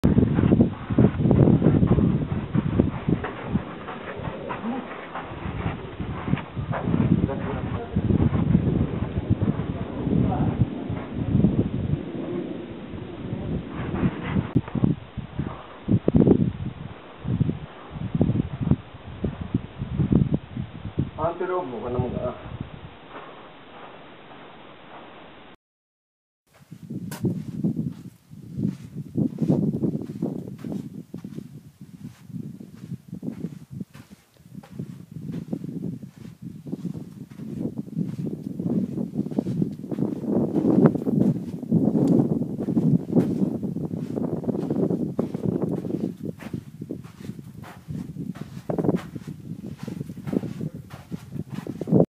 Kasha-Katuwe Tent Rocks Walking about 15 miles away from Route 66
Kasha-Katuwe Tent Rocks National Monument Walking about 15 miles away from Route 66, NM. U.S.A. on Jun. 23, 2012.